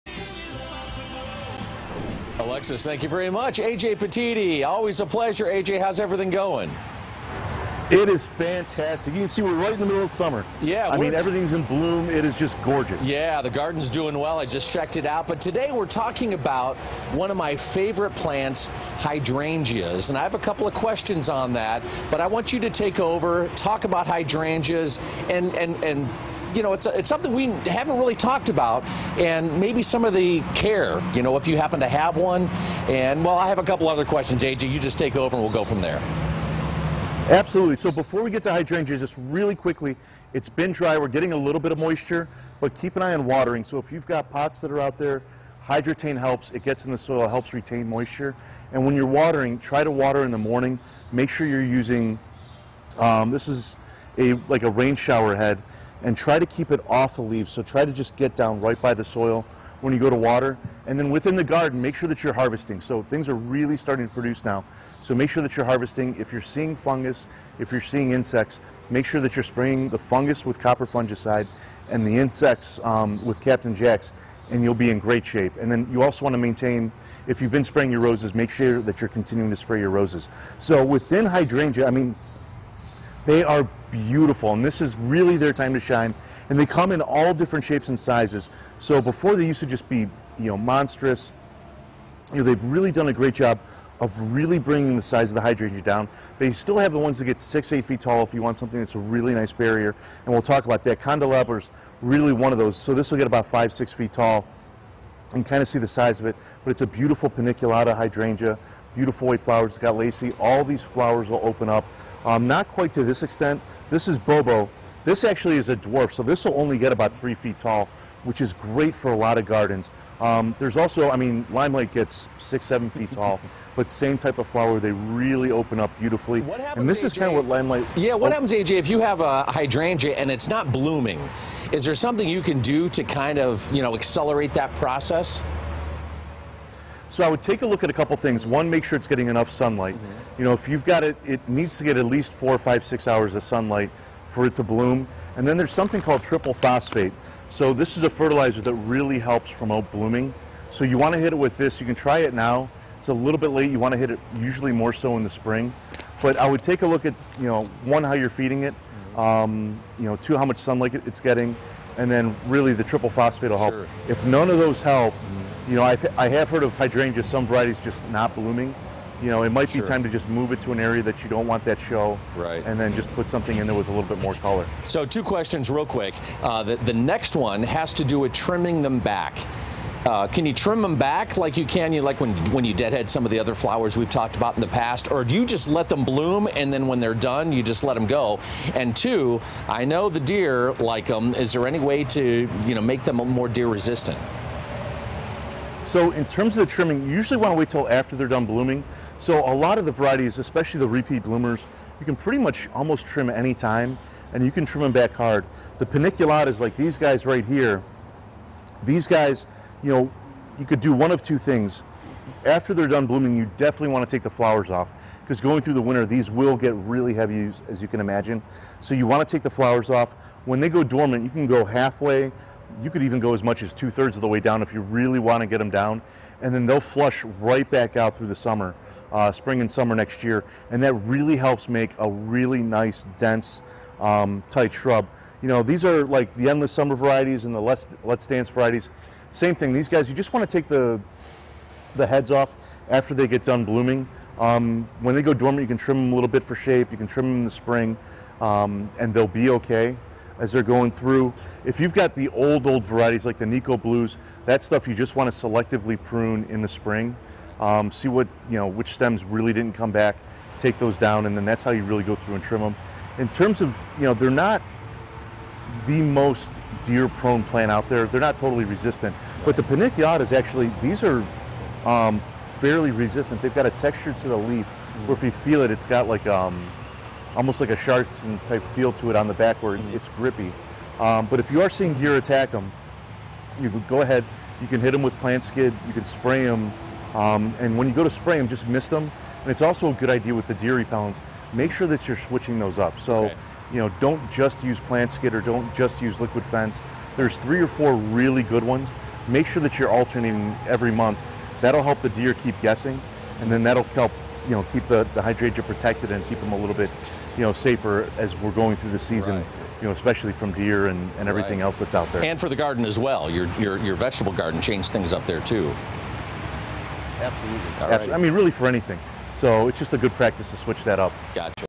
Dig This: Hydrangea Help from AJ Petitti
How are your hydrangea's doing this summer? Fox 8's Scott Sabol checked in with AJ Petitti, President of Petitti Garden Centers, to get some great tips on the best way to care for the popular Northeast Ohio shrub.